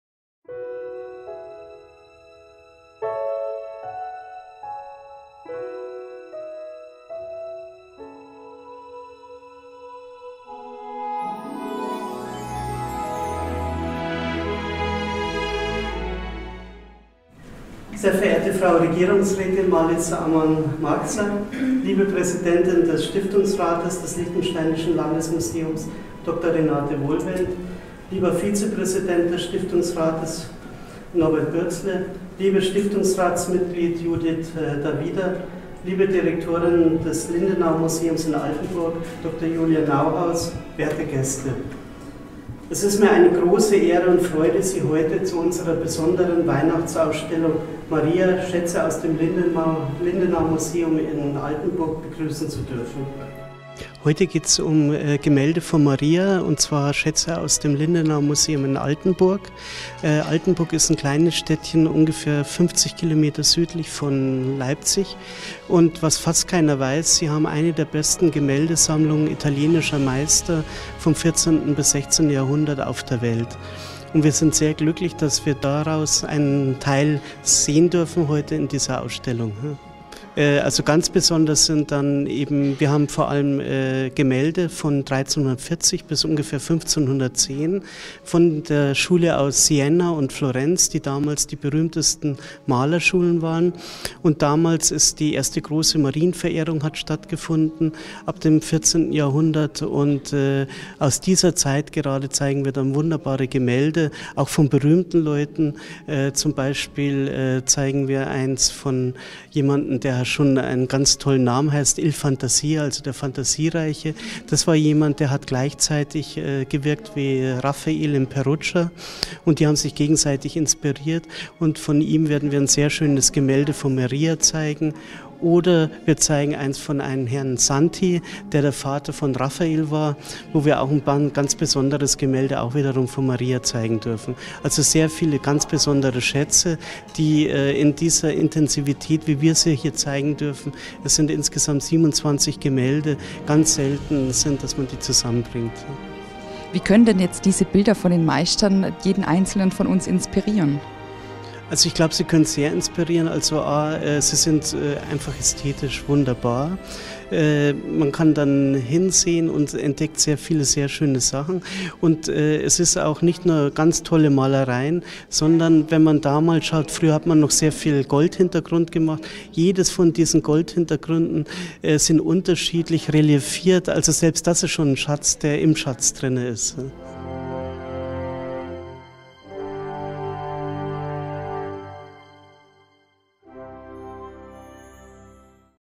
Maria-Landesmuseum
Vom 28.11. bis 06. Januar gibt es eine neue Sonderausstellung im liechtensteinischen Landesmuseum. Herr Rainer Vollkommer präsentierte die Weihnachtsausstellung, Maria – Schätze aus dem Lindenau-Museum in Altenburg, mit einer Führung durch die bildlich dargestellten Kunstwerke…